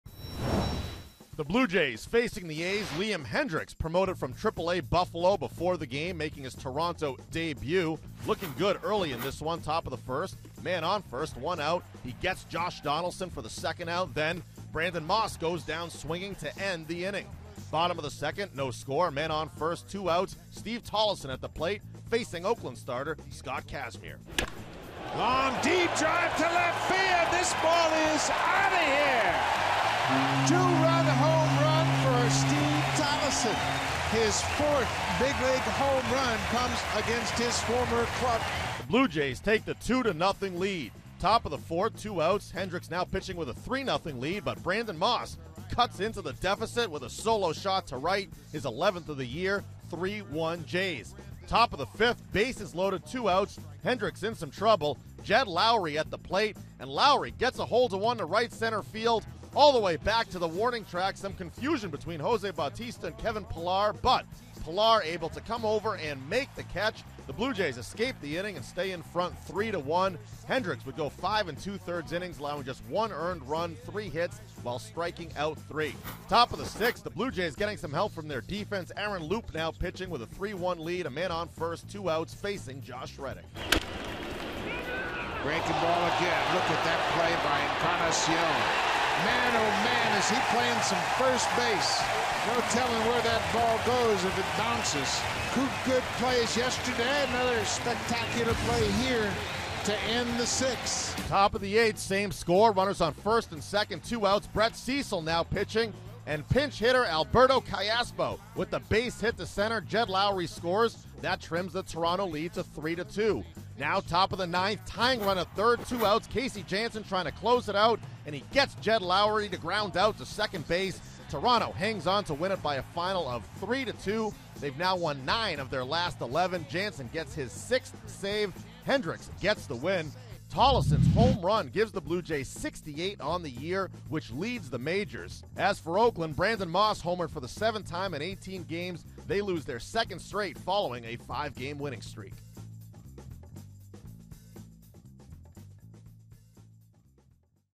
5/23/14: Hendriks earns a win in his season debut
Daily Recap: Liam Hendriks held the A's to one run over 5 2/3 in his first start of the season, while Steve Tolleson blasted a two-run homer